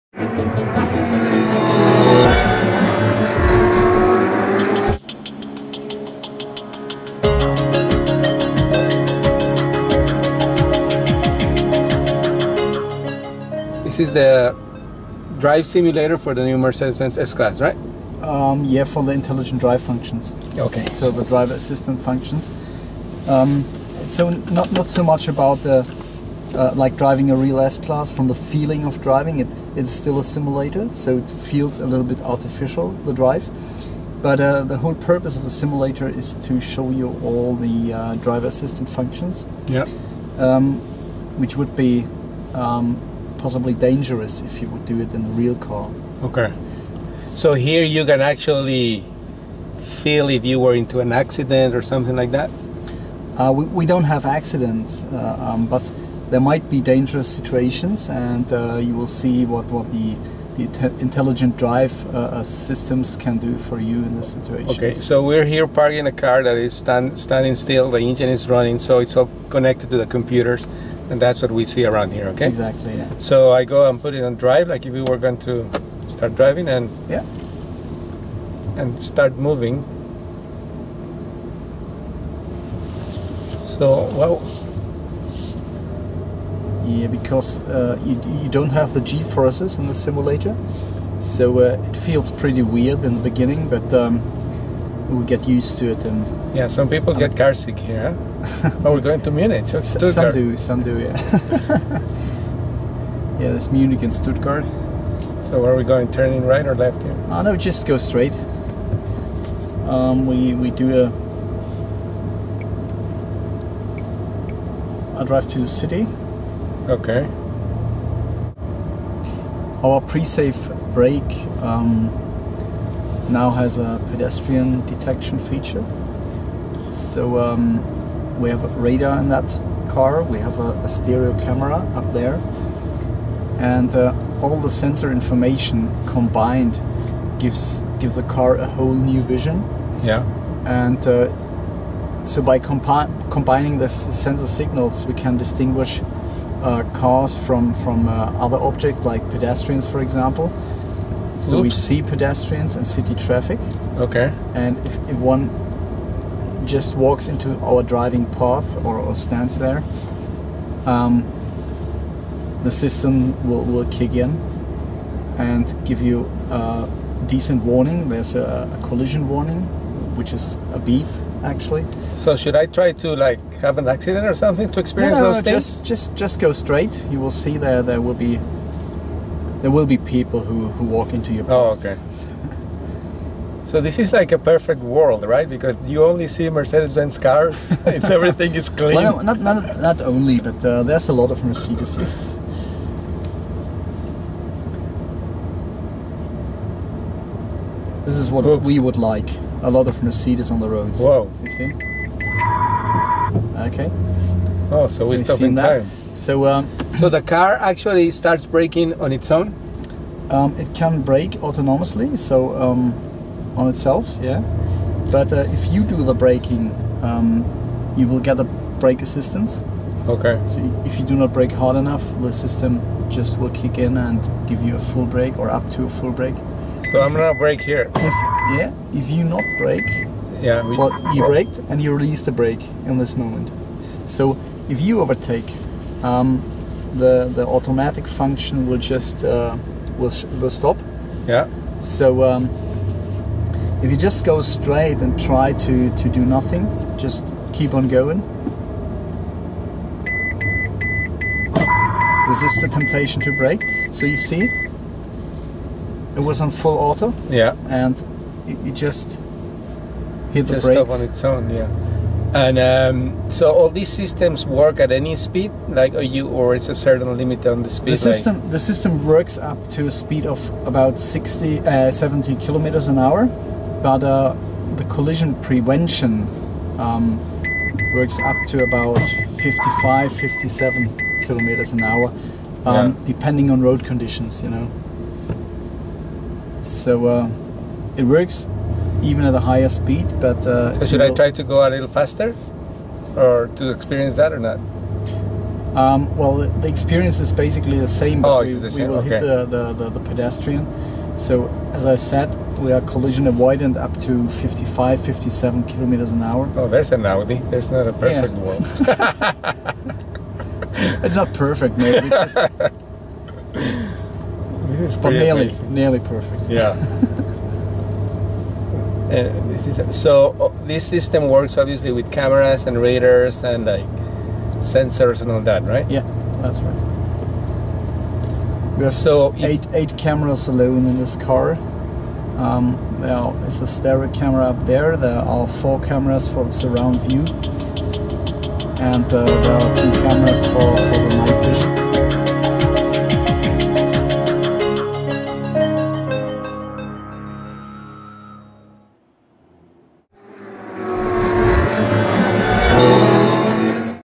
2014 Mercedes-Benz S Class Intelligent Drive Simulator
2014 Mercedes-Benz S Class Intelligent Drive Simulator

What started with PRE-SAFE® ten years ago and continued with DISTRONIC PLUS has now resulted in a new dimension of motoring: comfort and safety are merged into one. Mercedes-Benz refers to this as "Intelligent Drive". A whole host of new systems makes the new S-Class even more comfortable and even safer.